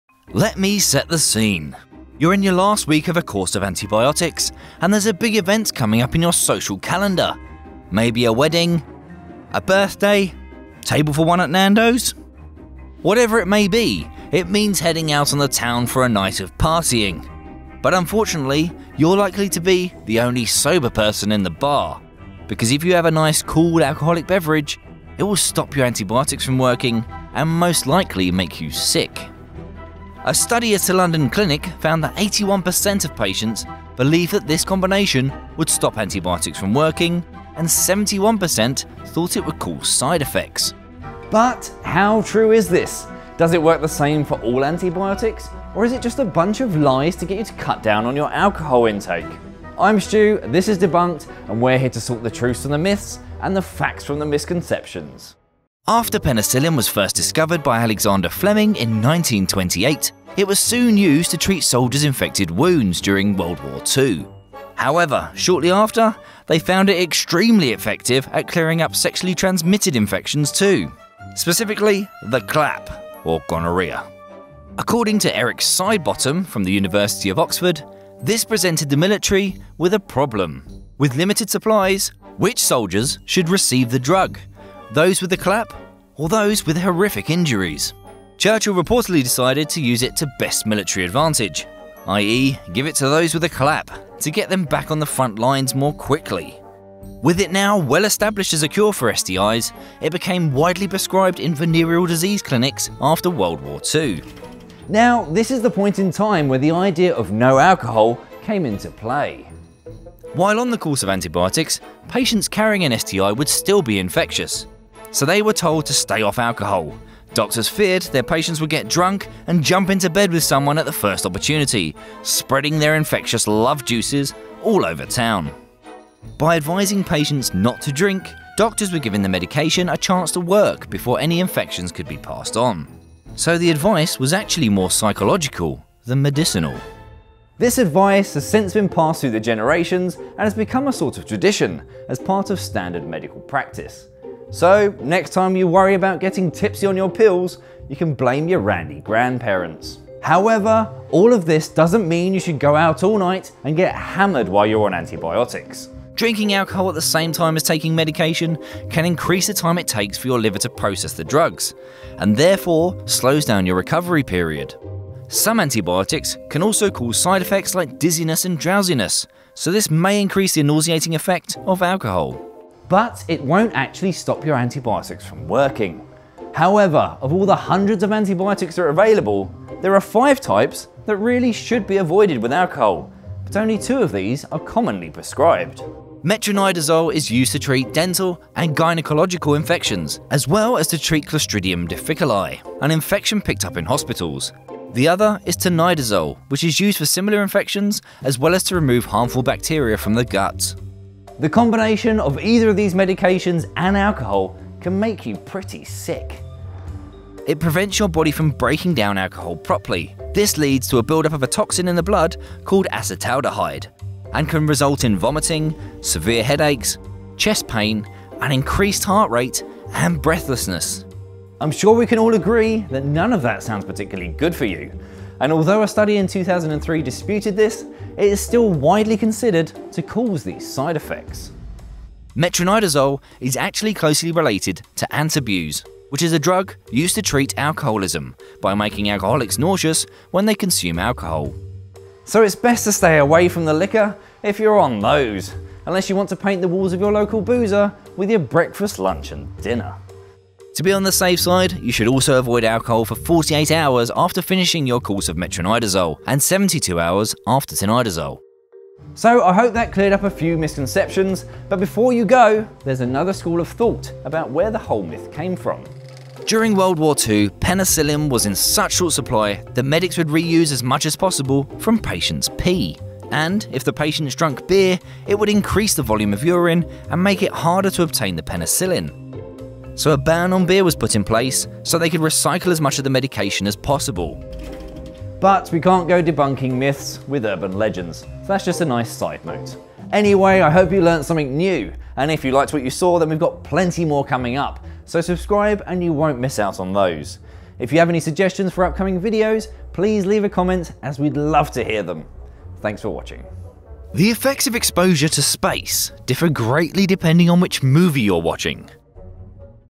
Can You Drink With Antibiotics? DEBUNKED
A study found that most people believe that mixing antibiotics with alcohol will either stop the medication working or make you sick. But what really happens?